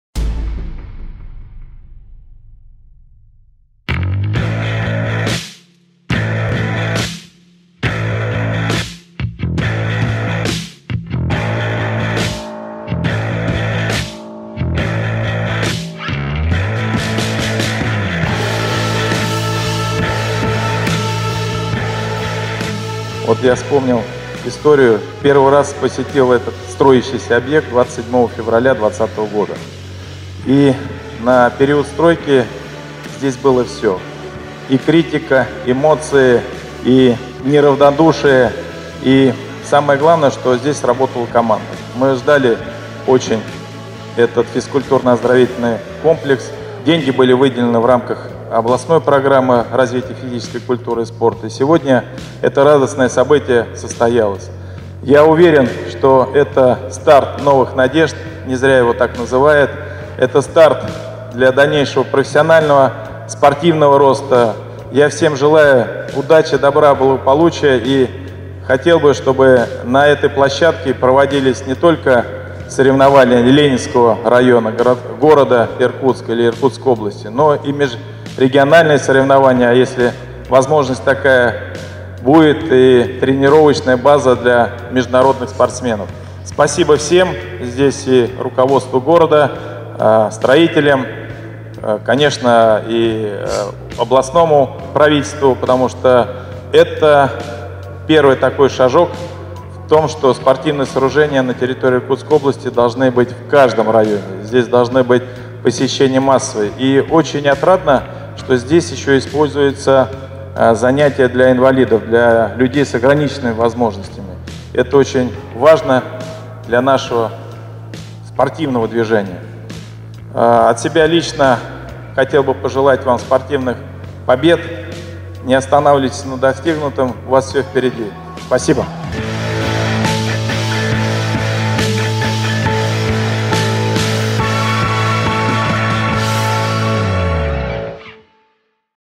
Физкультурно-оздоровительный комплекс «Старт» открылся в Иркутске
В Иркутске на улице Баумана открылся физкультурно-оздоровительный комплекс «Старт». Об этом 9 февраля 2021 года сообщили в пресс-службе правительства региона.

Строительство в январе 2020 года в рамках областной государственной программы «Развитие физической культуры и спорта». Общая стоимость объекта составила более 71 млн рублей. Деньги в равных долях были выделены из областного и местного бюджетов.

– Первый раз я посетил этот строящийся объект 27 февраля прошлого года. Мы очень ждали этот физкультурно-оздоровительный комплекс. И сегодня это радостное событие состоялось. Я уверен, что это старт новых надежд, это старт для дальнейшего профессионального спортивного роста. Хотелось бы, чтобы на этой площадке проводились не только соревнования Ленинского района, города Иркутска и Иркутской области, но чтобы она могла стать тренировочной базой для спортсменов-международников, - сказал Игорь Кобзев.

По проекту открытие физкультурно-оздоровительного комплекса планировалось 1 сентября 2020 года. Однако из-за задержек подрядчика срок был перенесен. На базе ФОКа «Старт» будет работать отделение по баскетболу муниципальной спортивной школы «Спартак», в котором смогут заниматься около двухсот детей.

На первом этаже  расположен универсальный игровой зал, на втором этаже – тренажерный зал. Еще в здании есть медицинский кабинет, разные административные и вспомогательные помещения. ФОК смогут посещать и люди с ограниченными возможностями, для этого установлен пандус, лифт, есть специальные раздевалки.

За зданием физкультурно-оздоровительного комплекса обустроена открытая спортивная площадка для занятий воркаутом.